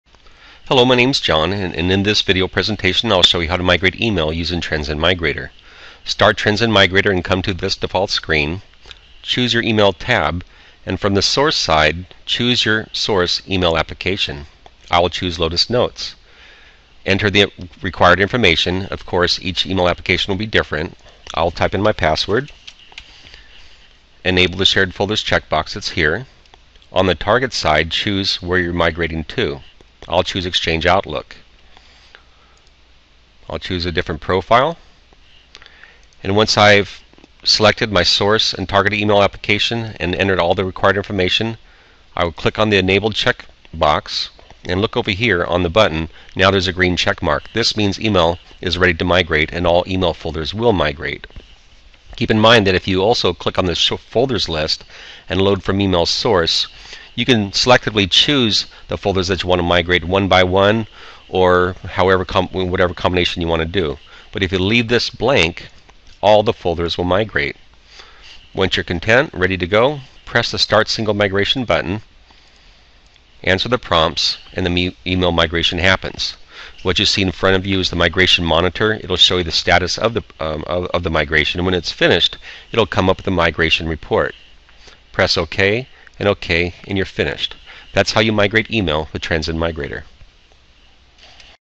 Email Migration with Transend Migrator
This video demonstrates how to migrate email with Transend Migrator. You can migrate email separately, or with Address Books, Calendars, and Tasks.